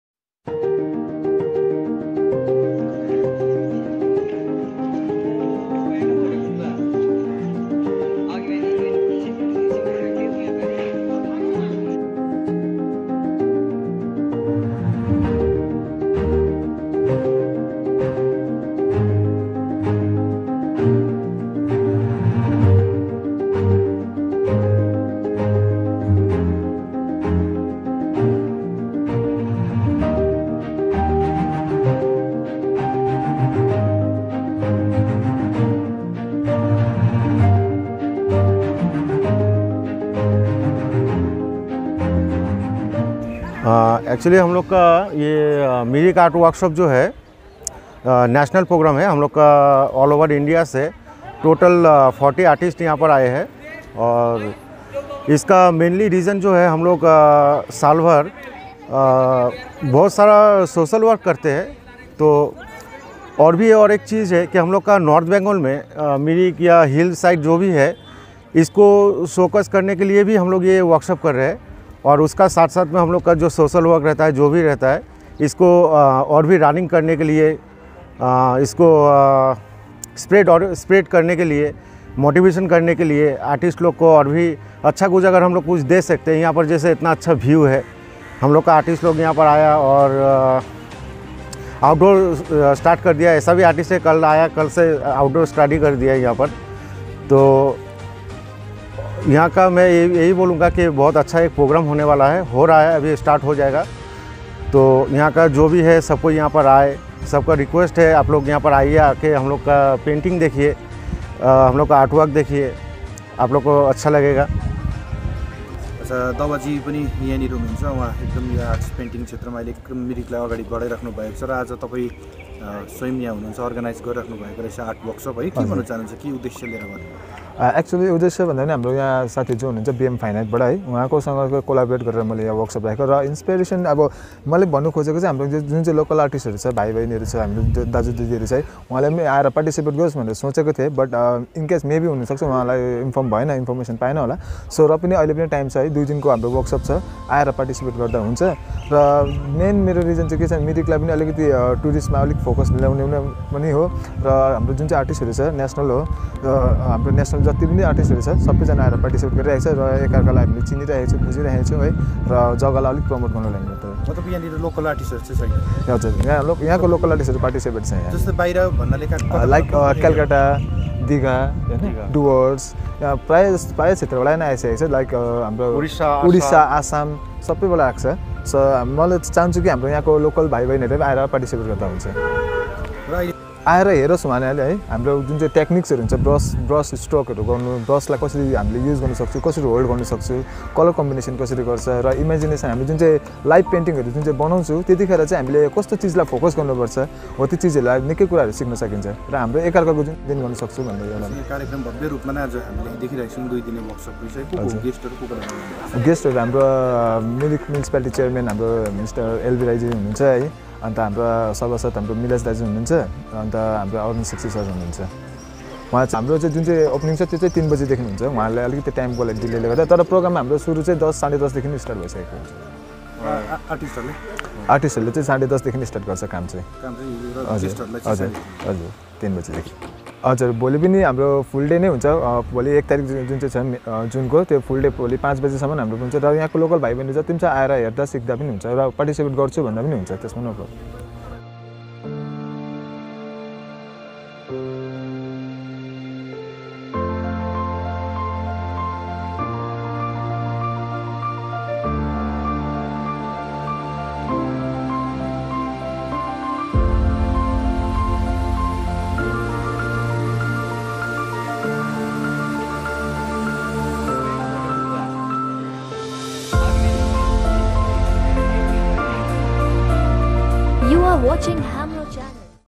FInE Arts Two days fine Art worshop
Two days fine Art worshop in Mirik

BM fine art (BMFAC) in collaboration Siliguri Rang tuli and Mirik canvas.com havw organised two days Fine Art  workshop in Mirik lake premises.

Dawa Lama founder of canvas.com Mirik and proprietor of Siliguri Rang tuli Abisekh Basunia addressing media.